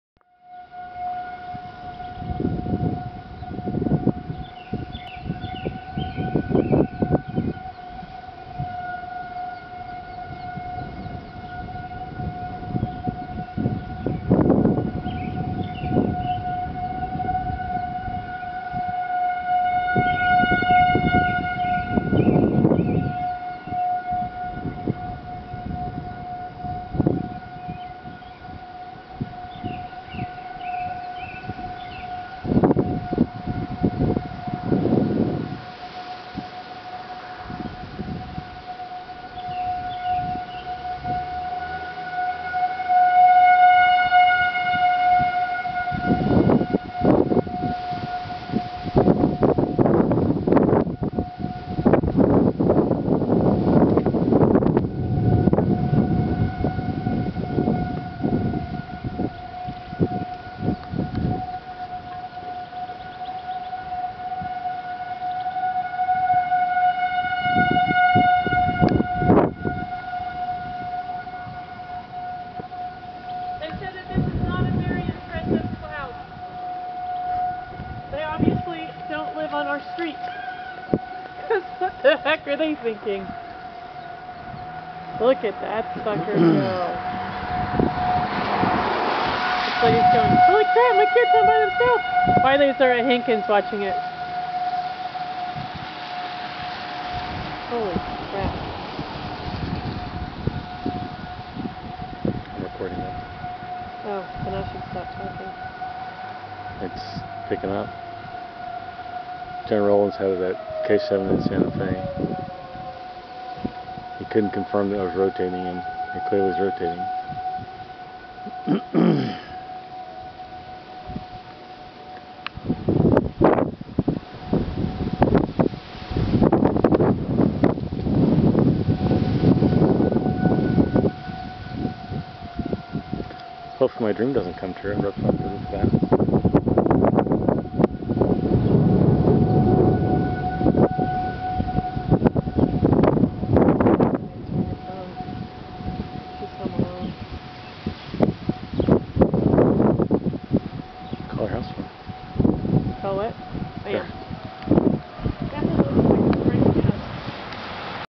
Funnel Cloud, Olathe, KS (5-6-12).AVI
My co-worker shot this video of a pretty impressive funnel cloud looming over his neighborhood yesterday evening. Fortunately, the weather system was just too weak to turn this funnel into a bonafide tornado. Good thing too, or my co-worker may not have had a home left today.